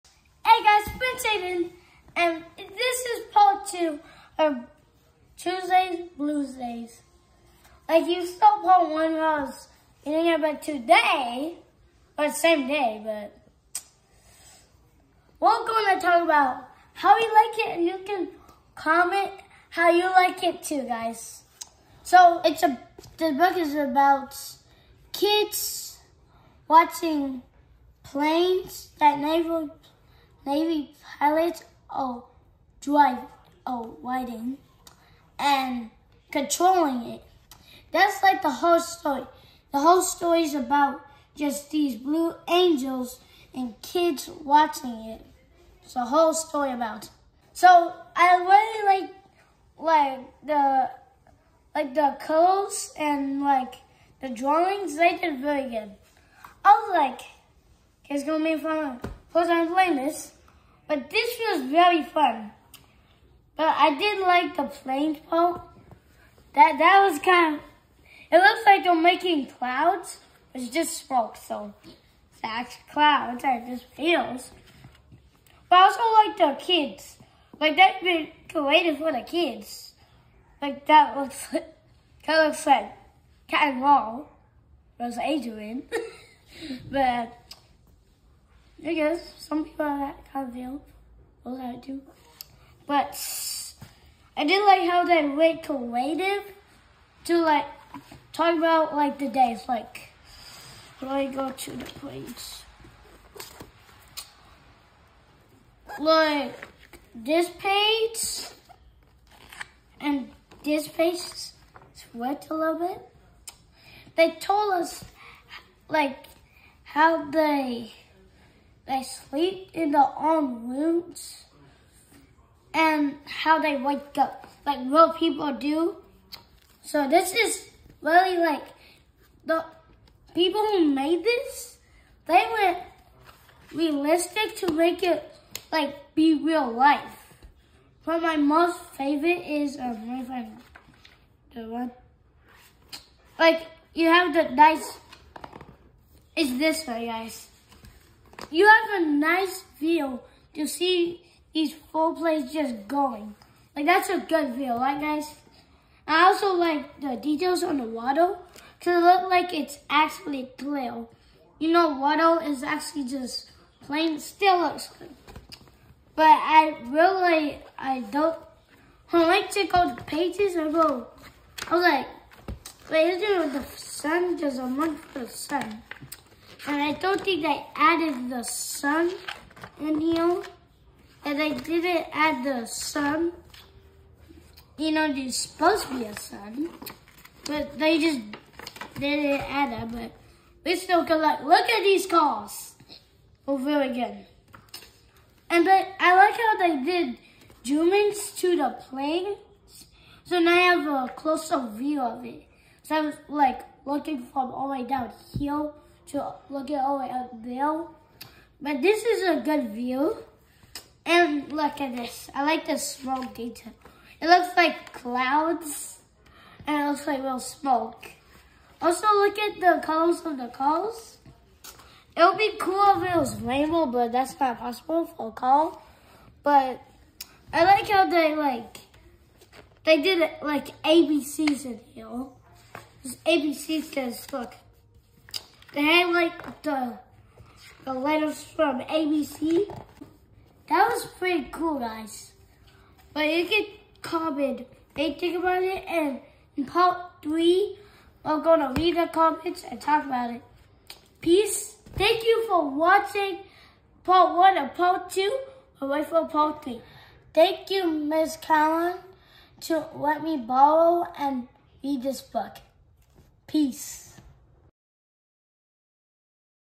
Book Review Part2: Tuesday Bluesday by Angel Stull-James
Aiden describes in his words what the book Tuesday Bluesday is about and some things he liked about it.